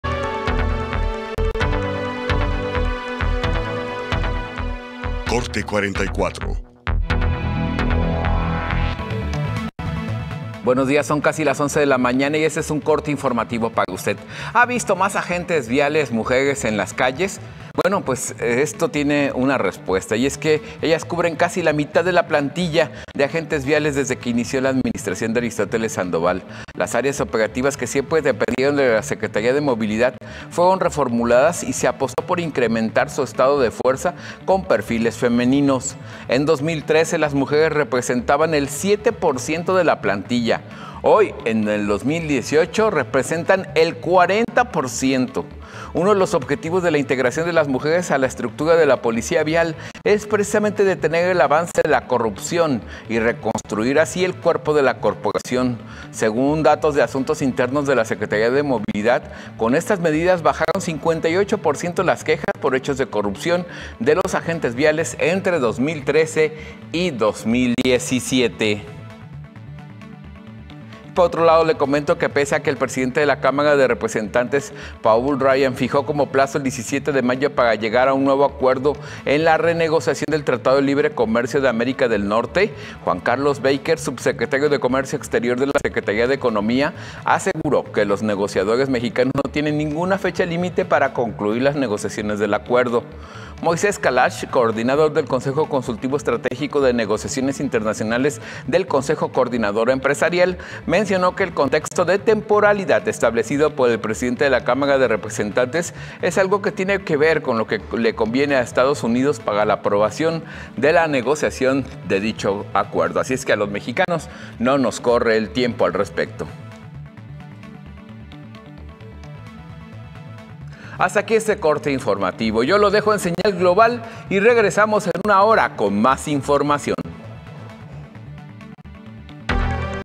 Corte 44 | ⚡️ Mujeres cubren casi la mitad de plantilla de vialidad desde que gobierna Aristóteles
⚡️ Mujeres cubren casi la mitad de la plantilla de agentes viales desde que inició la administración de Aristóteles Sandoval, las áreas operativas que siempre dependieron de la Secretaría de Movilidad (Semov) fueron reformuladas y se apostó por incrementar su estado de fuerza con perfiles femeninos.

⚡️ Pese a que el presidente de la Cámara de Representantes, Paul Ryan, fijó como plazo el 17 de mayo para llegar a un nuevo acuerdo en la renegociación del Tratado de Libre Comercio de América del Norte (TLCAN), Juan Carlos Baker, subsecretario de Comercio Exterior de la Secretaría de Economía, aseguró que los negociadores mexicanos no tienen ninguna fecha límite para concluir las negociaciones del acuerdo.